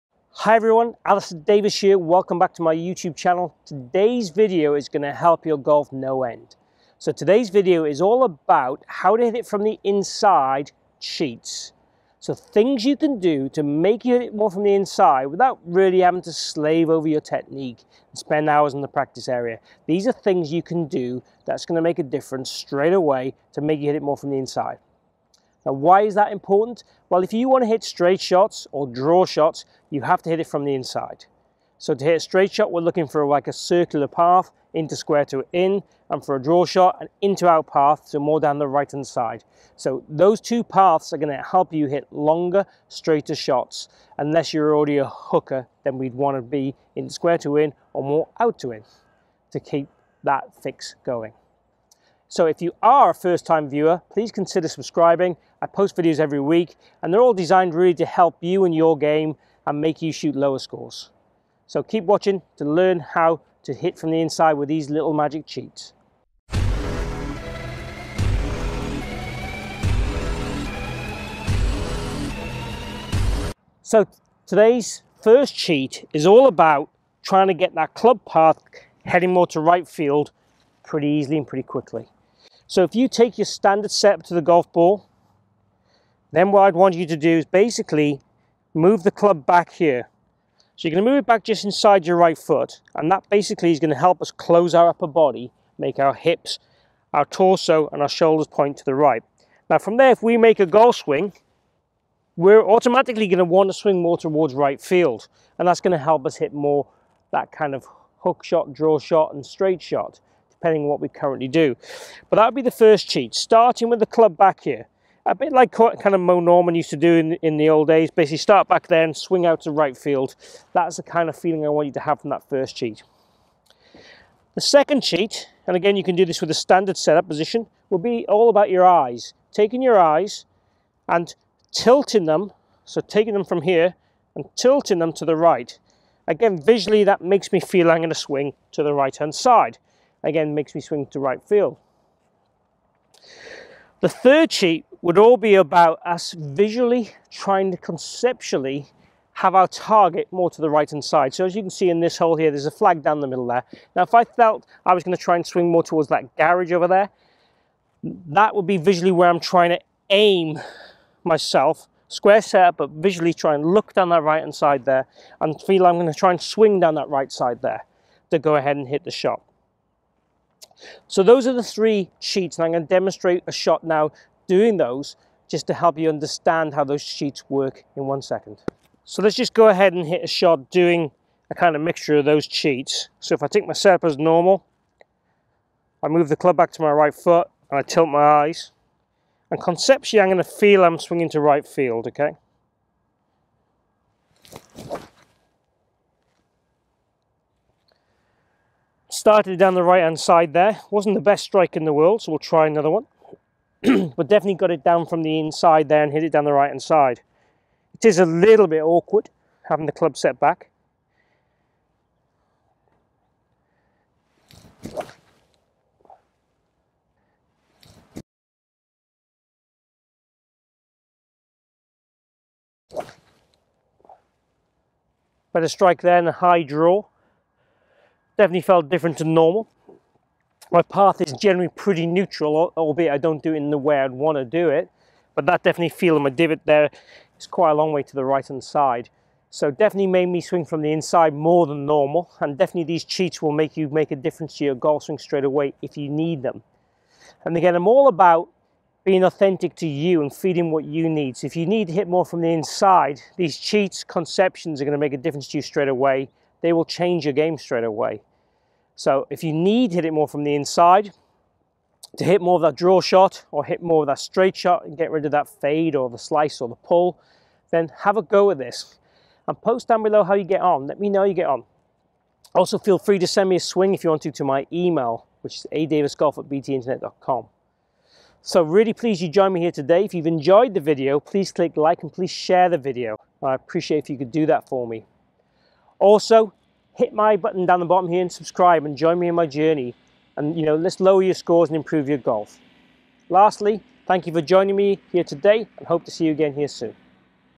How To Hit It From The INSIDE In Golf (Cheats)
Alistair Davies golf uk top 50 golf coach shares with you how to swing from the inside consistently.
Hit straight shots and draws with this method. If you slice or fade the ball this method will help.
These cheats provide instant results that will help improve your scores.